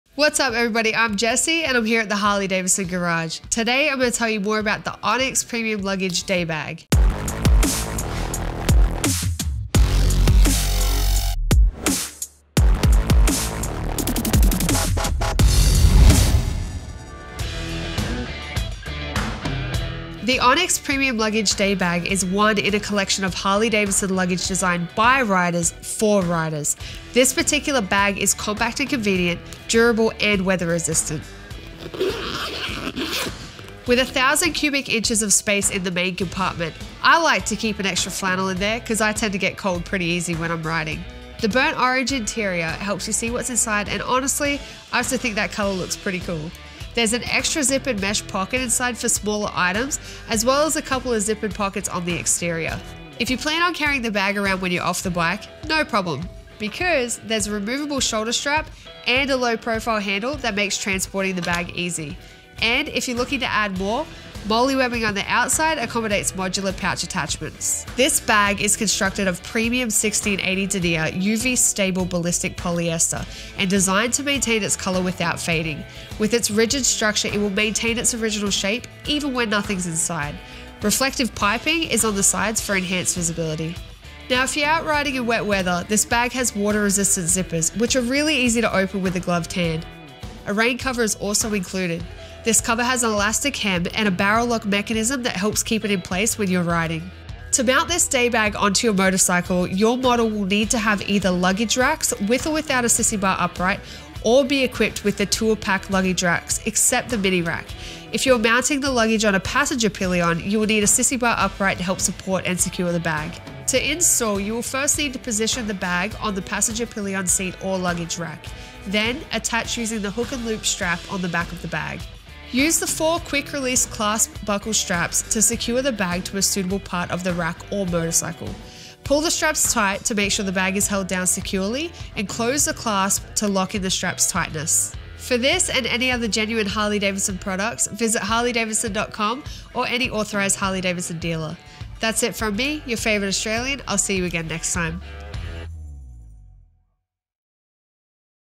Harley-Davidson Onyx Premium Luggage Day Bag Overview
Designed by riders for riders, the Harley-Davidson Onyx Premium Luggage Collection is the best luggage Harley-Davidson has to offer and provides everything you need in motorcycle luggage and nothing you don't.

This Harley-Davidson Onyx Premium Luggage Day Bag is perfect for your daily work commute or a short day trip. The main compartment is great for storing a sweatshirt, a pair of shoes, a rainsuit, gloves, and cap. The interior mesh zip pocket holds keys or small electronics.

The rigid structure of the bag maintains its shape even when empty and the detachable padded shoulder strap makes the bag easy to carry. The bag also includes a rain cover featuring an elastic hem and barrel lock mechanism that cinches tight ensuring it stays in place at highway speeds.

Fits models equipped with Luggage Racks, with or without a Sissy Bar Upright, and Tour-Pak Luggage Racks (except Mini Rack P/N 53100-95). Installation requires a sissy bar upright if mounting luggage on a passenger pillion.